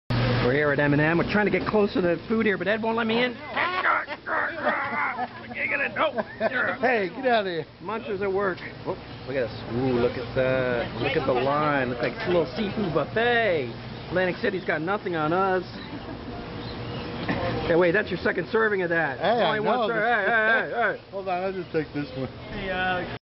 Stepping up to the bar
Ed Clancy and the rest of the ever-hungry Munch crew help themselves to takeout containers at M&M Steam Bar in  Beach Haven Terrace. Excellent fresh fresh here, especially the soups, sauteed softshell crabs and the blackened swordfish. (Video by Pete Genovese).